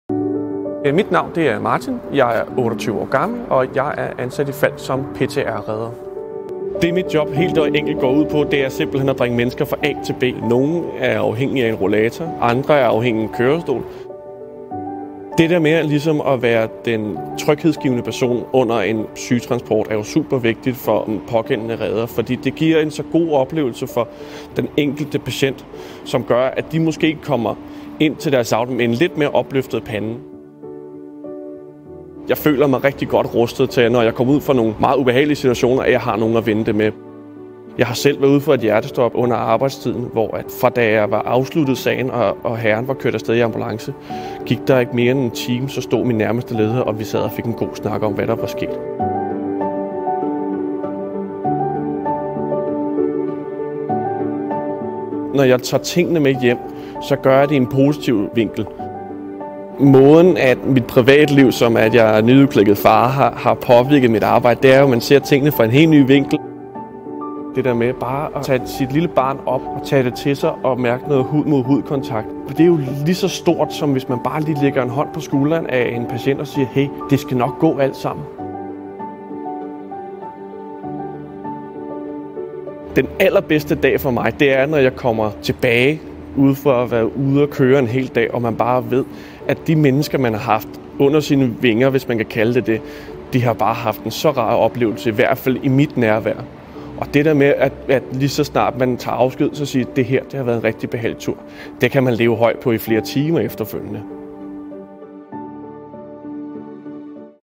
Mit Falck - Martin_UdenMundbind DK Subs
PTS redder Martin Juel Smidt fortæller om sit arbejde i Falck.